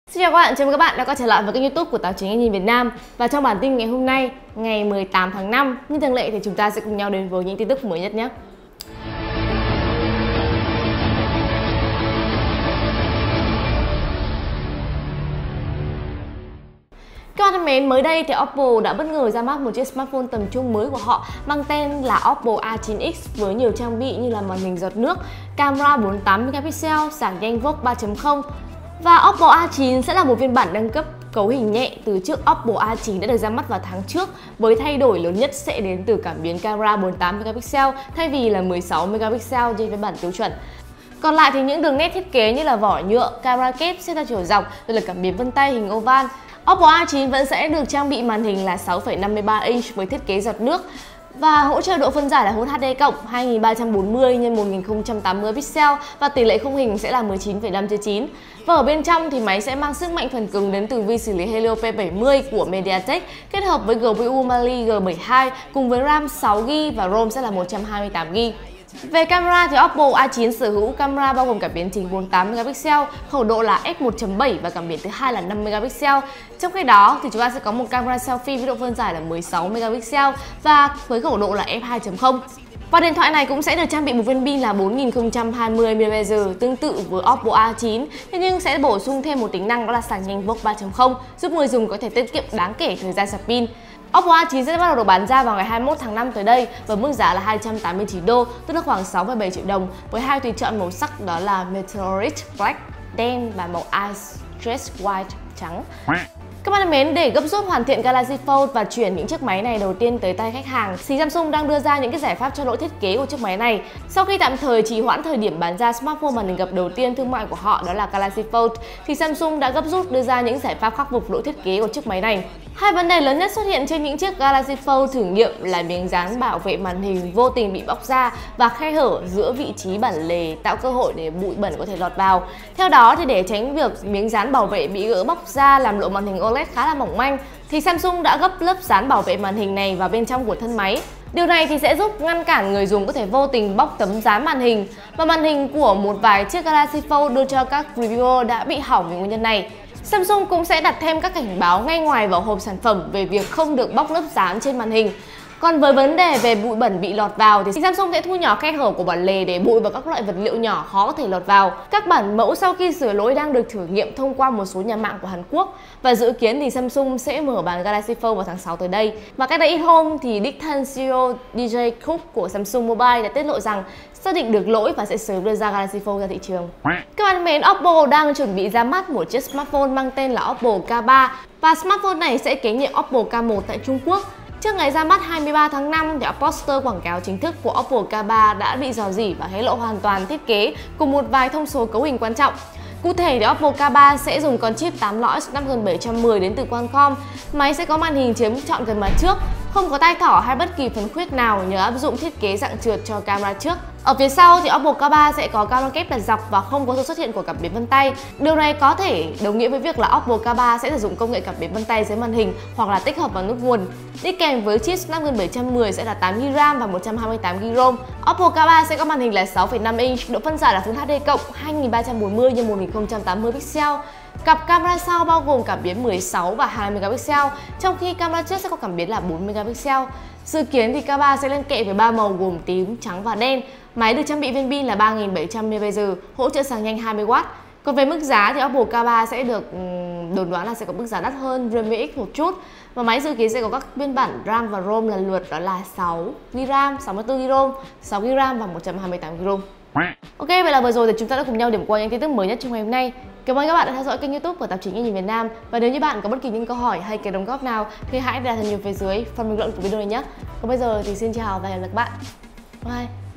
Oppo K3 camera trượt, Snapdragon 710 màn hình không khiếm khuyết
- Oppo A9X ra mắt: camera 48MP, sạc nhanh VOOC 3.0, giá 289 USD
- Samsung khắc phục lỗi thiết kế của Galaxy Fold
- Oppo K3 rò rỉ: màn hình không khiếm khuyết, camera trượt, Snapdragon 710